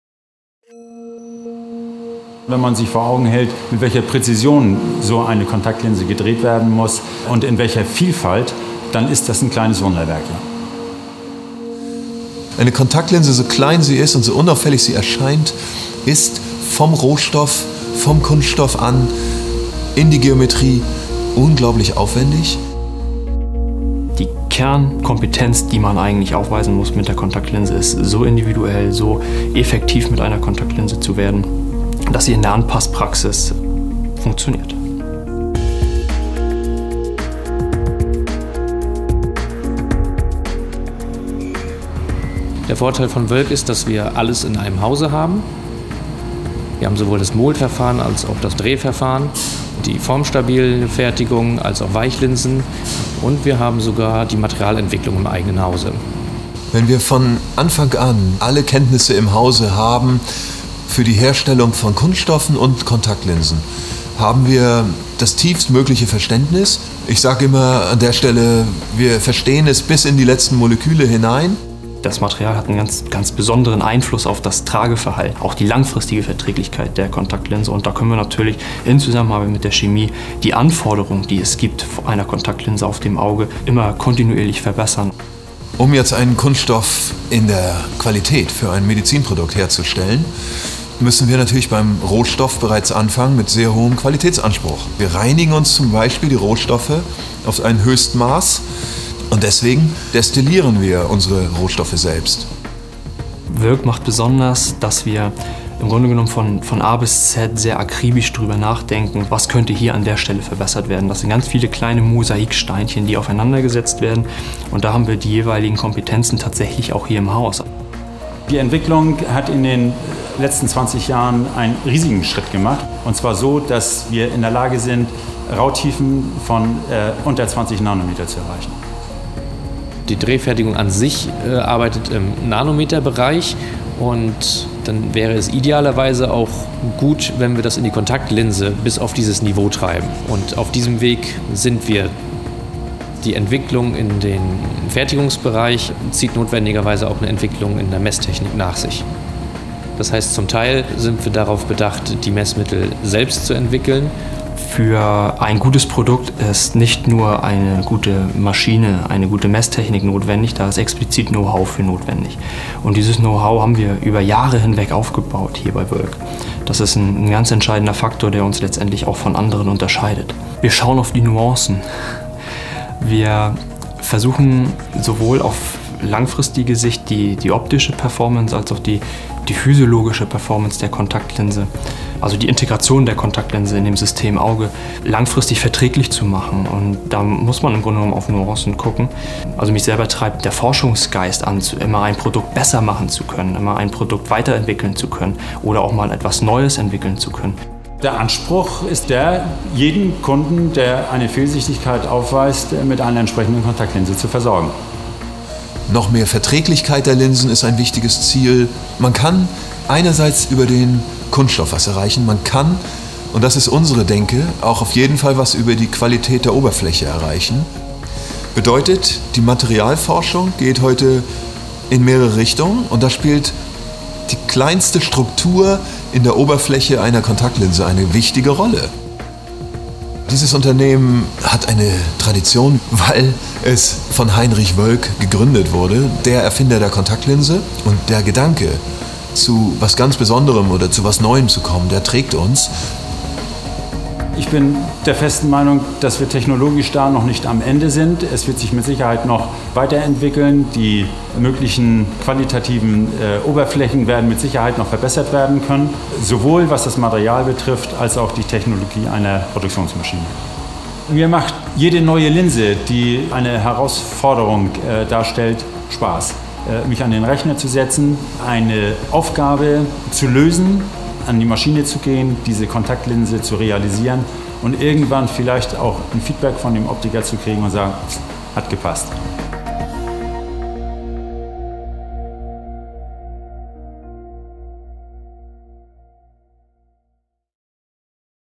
Wöhlk Contactlinsen – Einblicke in die Entwicklung und Produktion kleiner Wunderwerke
Wöhlk Contactlinsen, der deutschlandweite Marktführer im Bereich individueller Contactlinsen und renommierter Vorreiter bei der Entwicklung neuartiger und verträglicher Materialien, gibt Einblicke in die heutige hochtechnologische Conactlinsenentwicklung und -produktion. Sehen Sie sich jetzt den Film an und erfahren Sie vieles, was Sie noch nicht über Contactlinsen wussten!